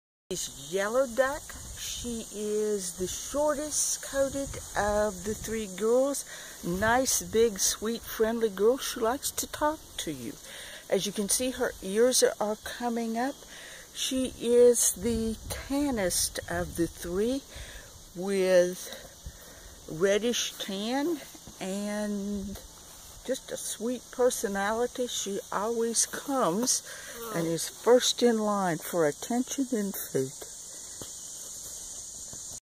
MookieGirl 2015 Aug01 yellowduck
Mookie & Falco 'Yellow Duck' girl born June 14. The video was taken August 1st and includes Emily's verbal description of the pup.